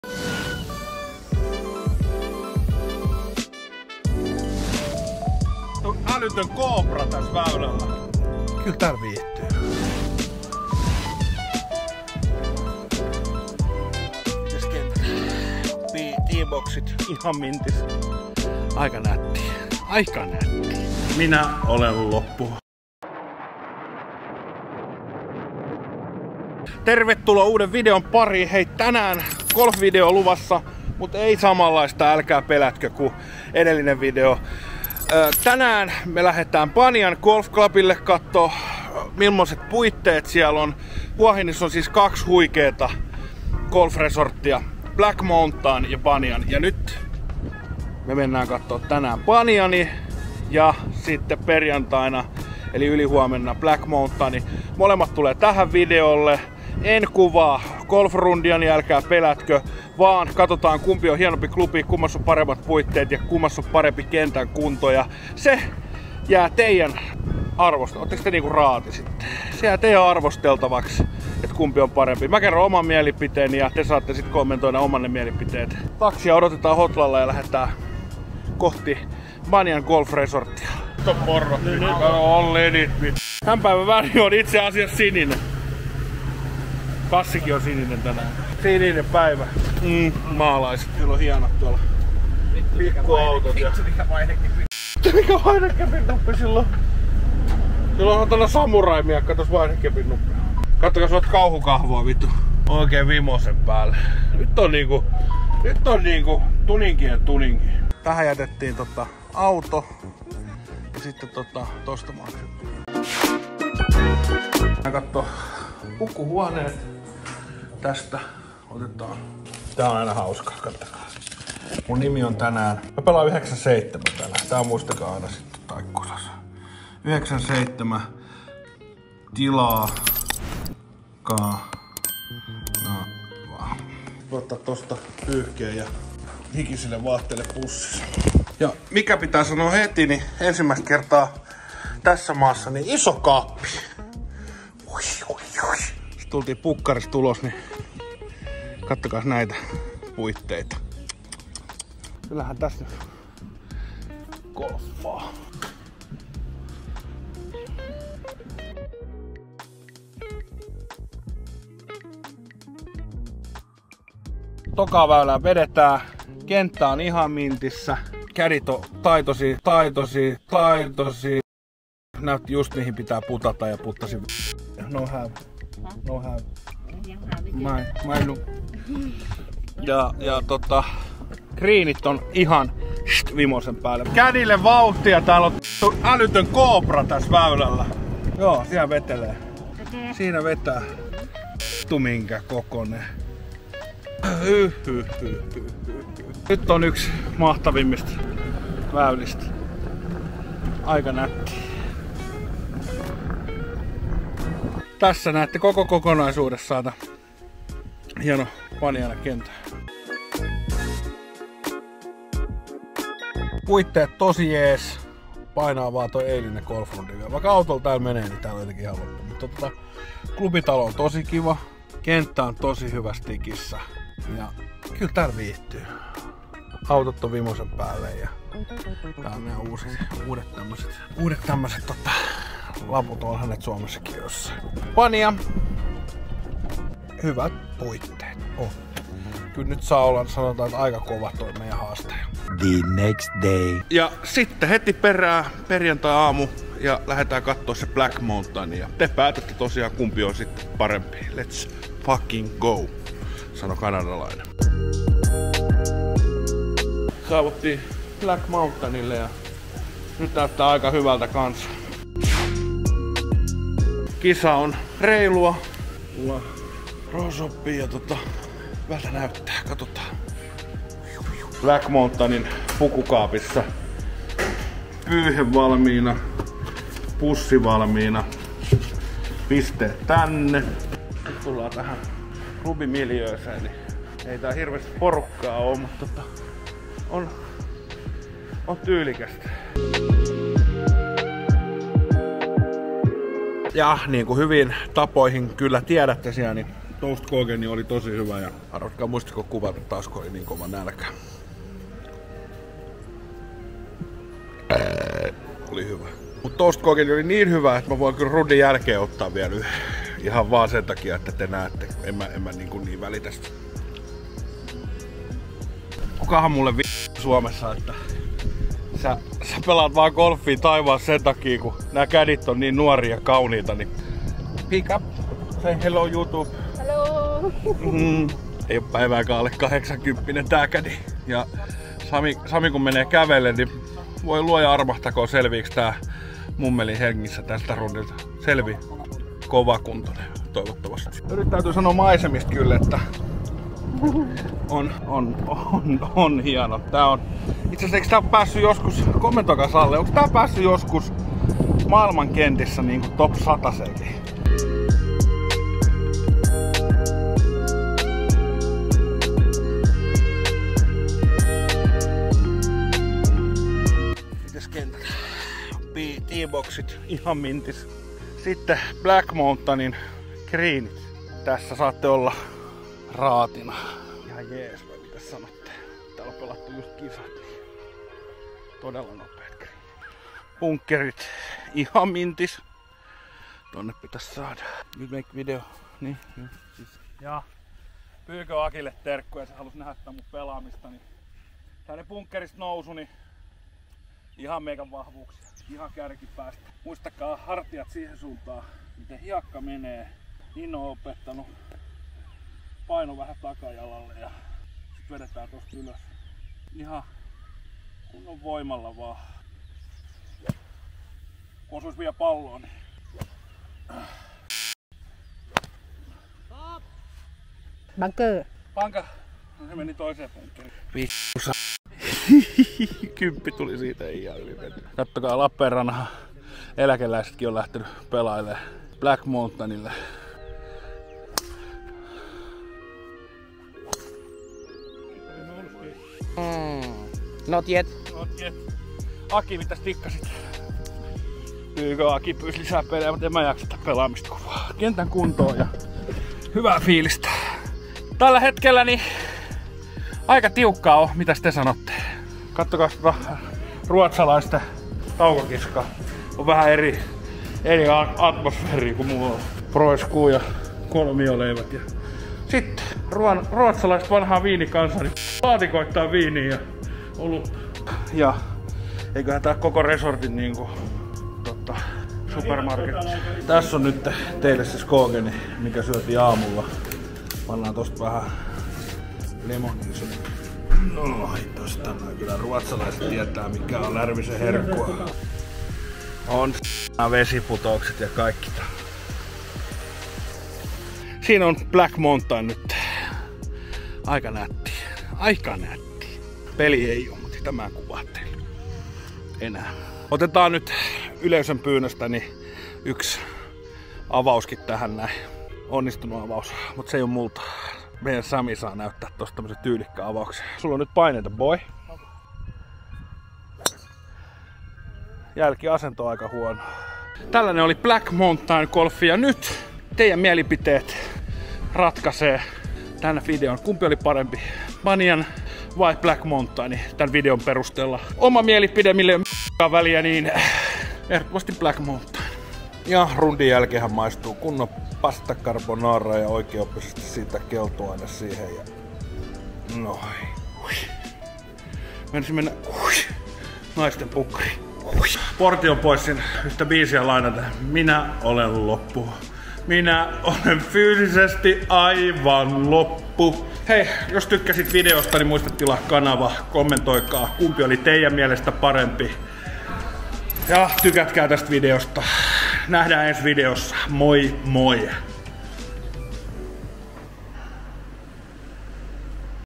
BANYAN & BLACK MOUNTAIN, KUMPI ON HUA HININ YKKÖNEN?
Käytiin pelaamassa Hua Hinin ykkös kentät eli Banyan ja Black Mountain. Kumpi on teidän mielestä parempi?